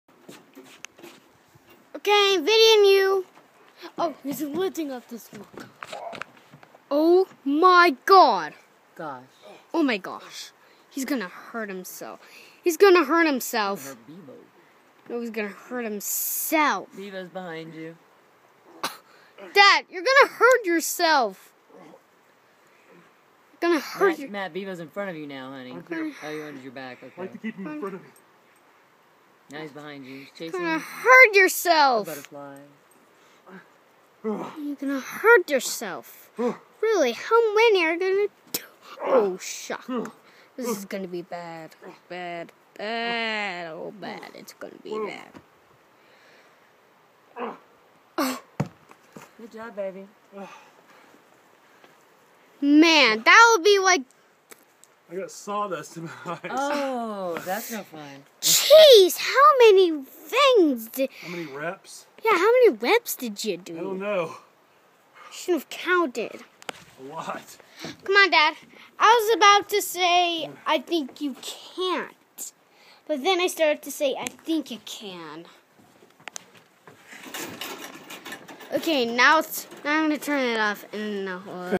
Matt's caveman lifting
Can't get to the gym?  Lift giant logs.